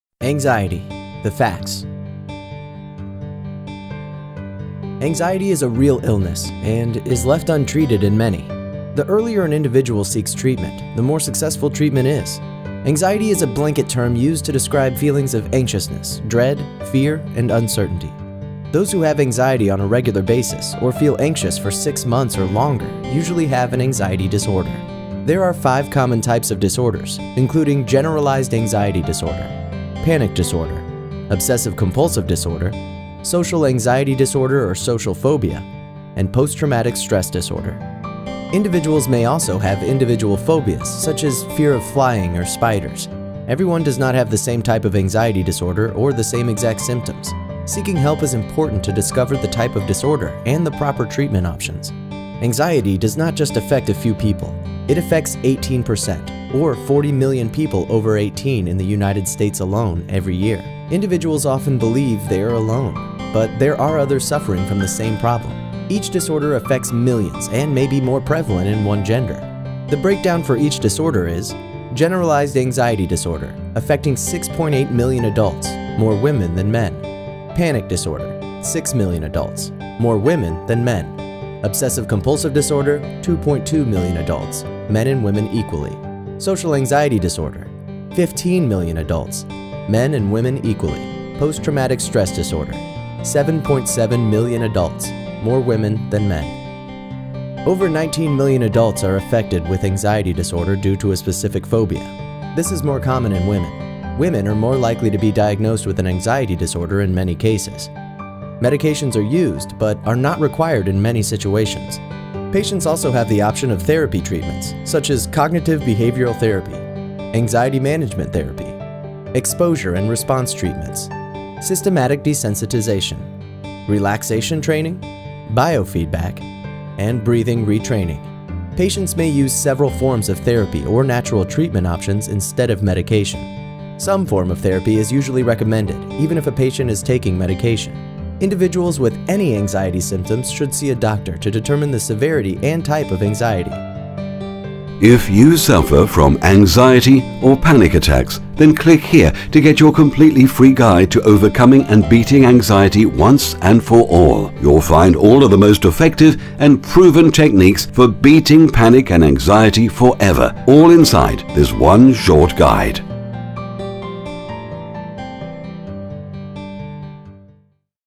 Anxiety Facts: The Must Know Facts About Anxiety
Many people are clueless when it comes to how anxiety works and many people would be shocked to know just how many people are affected by various anxiety disorders.

In this short 'Anxiety Facts' video you'll find all of the most important and interesting facts about anxiety disorders so that you can be gotten up to speed on how it works.

In short anxiety comes in many different forms including generalized anxiety disorder, panic disorder, obsessive compulsive disorder, social anxiety disorder (or social phobia) and post traumatic stress disorder.

Anxiety statistics show tha 18% or 40 million people in the United States are affected by anxiety disorders.

Furthermore more anxiety disorder statistics show that not all types of disorders effect men and women equally and some disorders can heavily lean to one way or the other.

For example social anxiety statistics show that both men and women to get it at the rate generalized anxiety disorder statistics on the other hand show that it affects women more than men.